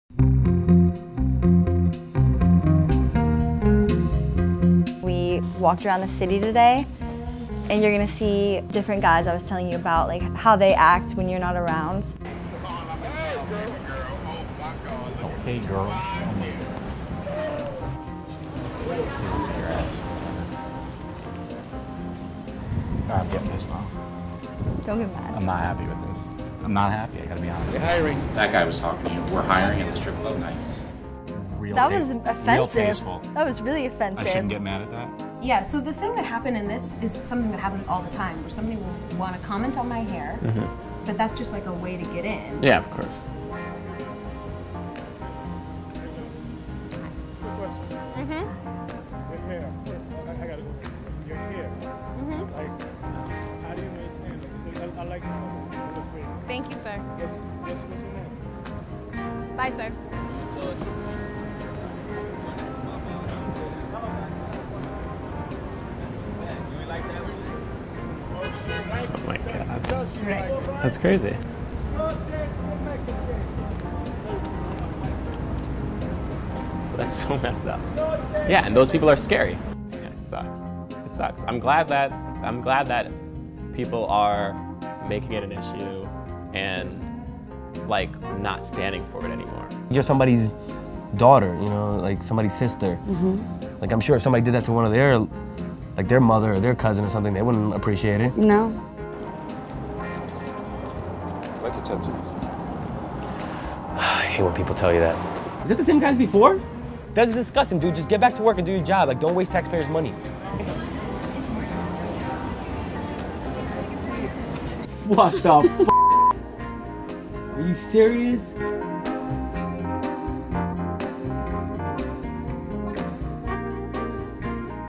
Men React to Their Girlfriends Getting Catcalled | Cosmopolitan
What happens when these boyfriends see their girlfriends getting catcalled on New York City streets?